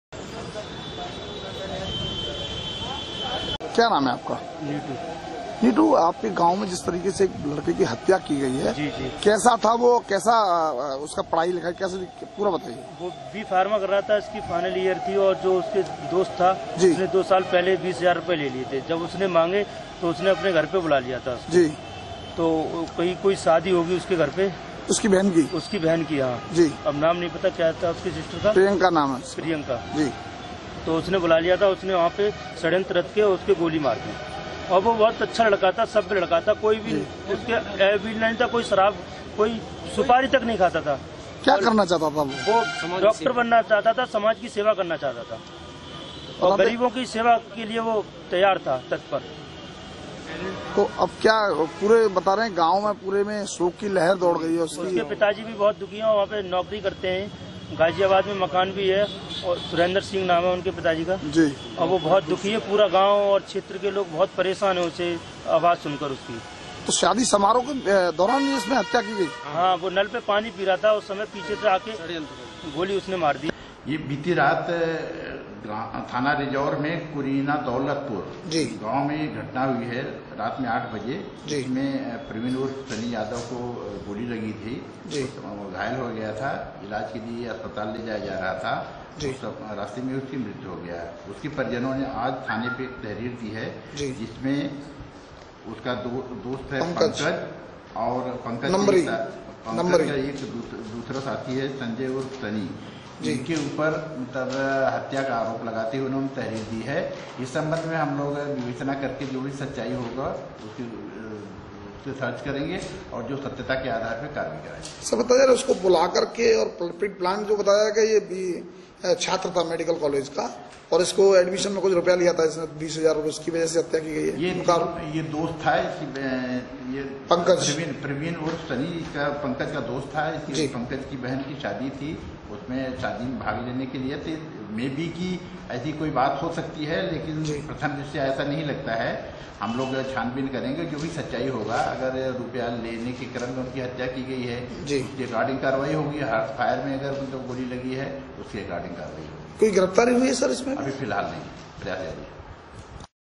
हर्ष फायरिंग में हुई युवक की मौत में आया नया मोड़, दोस्त पर लगा हत्या का आरोप
एटा: थाना रिजोर के गांव दौलतपुर कुरीना में शादी समारोह के दौरान हर्ष फायरिंग के नाम पर छात्र की हत्या के मामले में नया मोड़ सामने आया है. छात्र प्रवीन यादव की हत्या के आरोप उसके ही दोस्त पंकज पर लगे हैं. छात्र की हर्ष फायरिंग में नहीं बल्कि रुपये न दे पाने की मजबूरी के चलते उसकी हत्या कर दी गई.